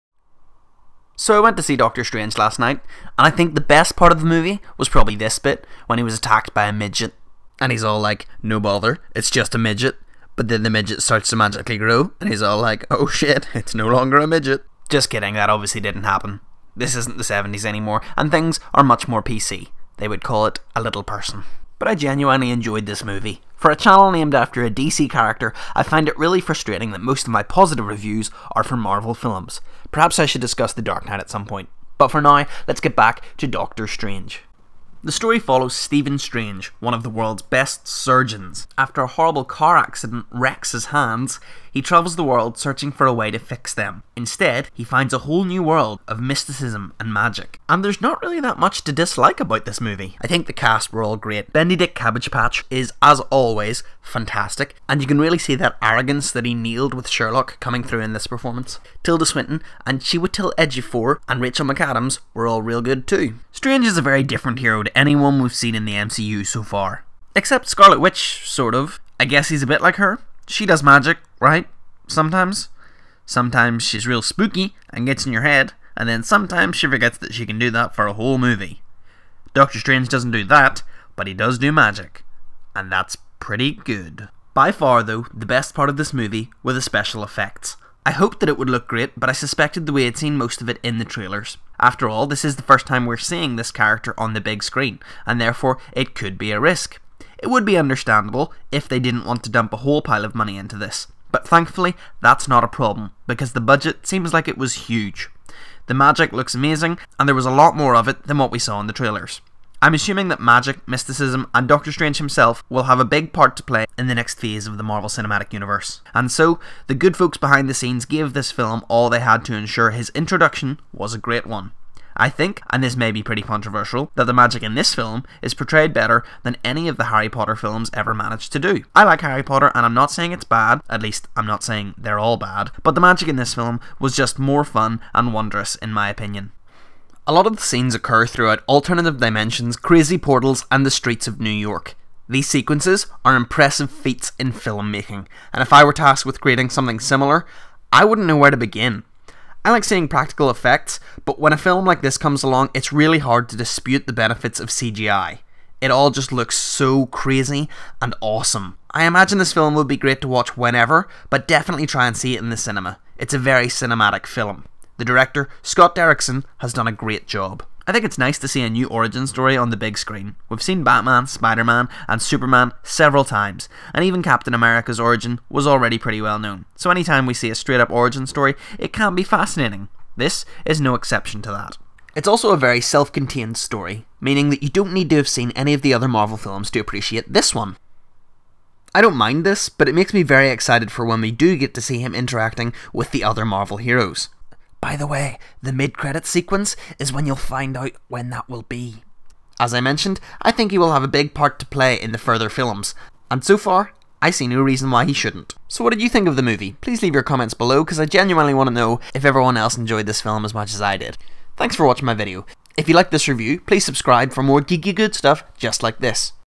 DOCTOR STRANGE Review
My review of the latest magical Marvel film.

All images and panels are owned by their respective companies.